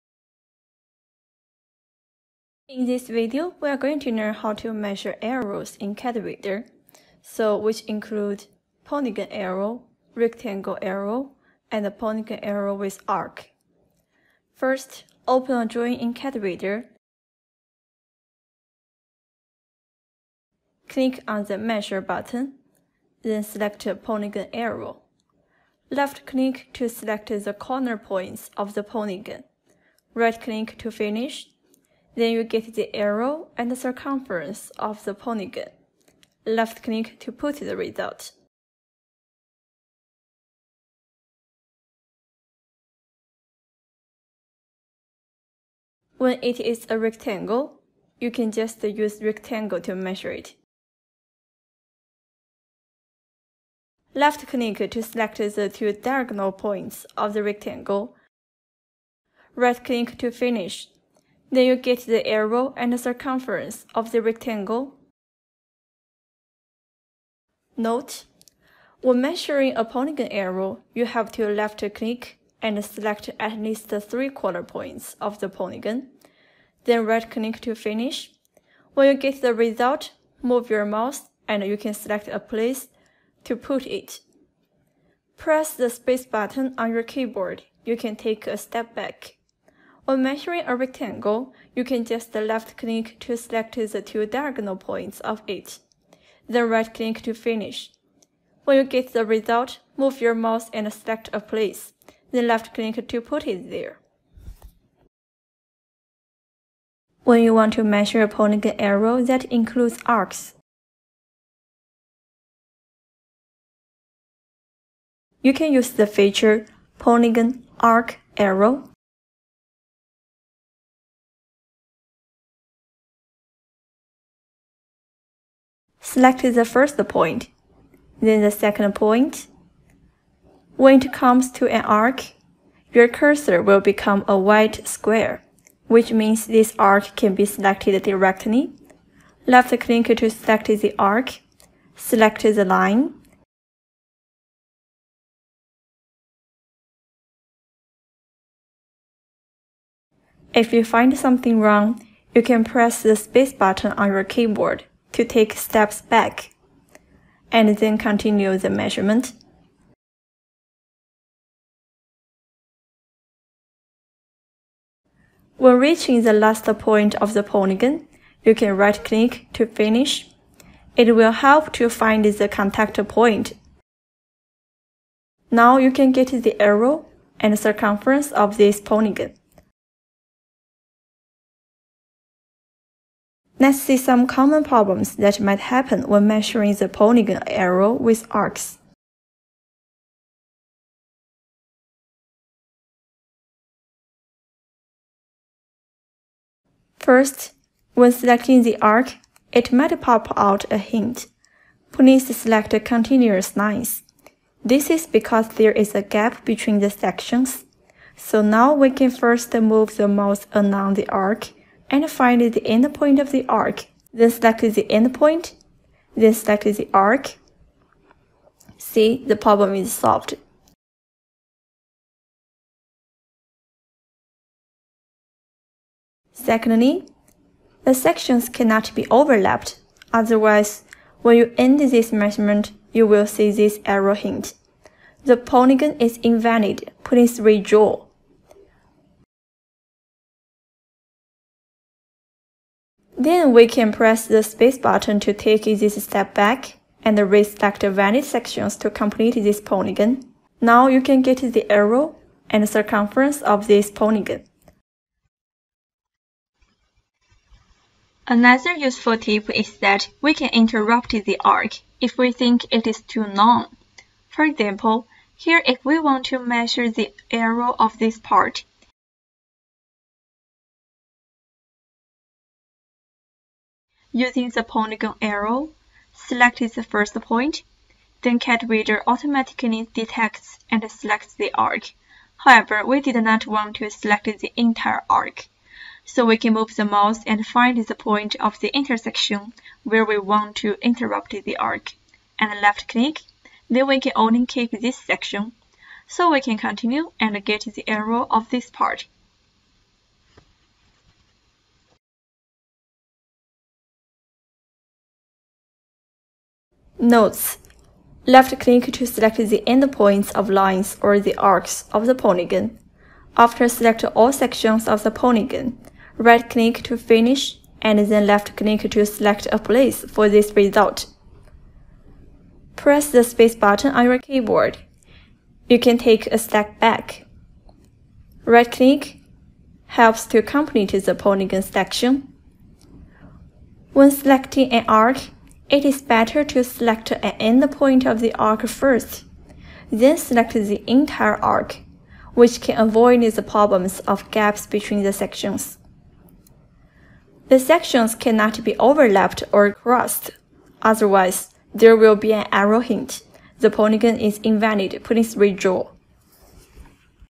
Measure Area of Shapes & Irregular Shapes
This tutorial video demonstrates on how to measure the area of different shapes such as polygon area, rectangle area, and also irregular shapes.

Available on PC, iOS and Android